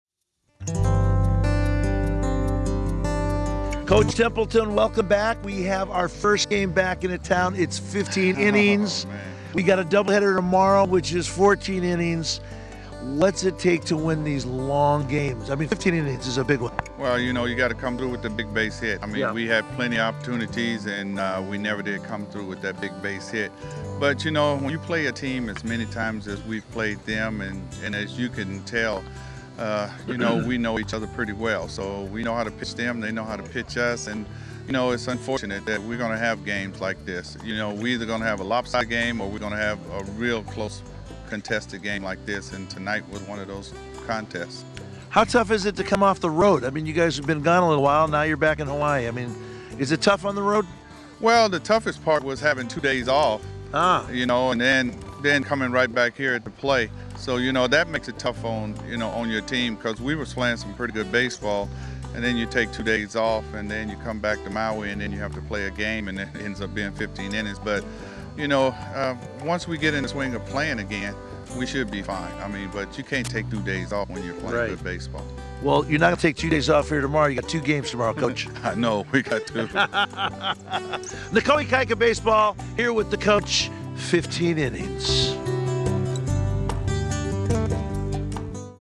15 Inning 6/18/11 - Garry Templeton team manager
6/18/11 - 15 Inning
Branscombe Richmond, interviews Na Koa Ikaika Baseball team manager, Garry Templeton at the Maehara Stadium, Maui Hawaii.

Produced and Directed by Branscombe Richmond for Richmond Productions. Camera and Edited by Nathan Perreira, Pacific Sunset Productions Maui (PSPMaui)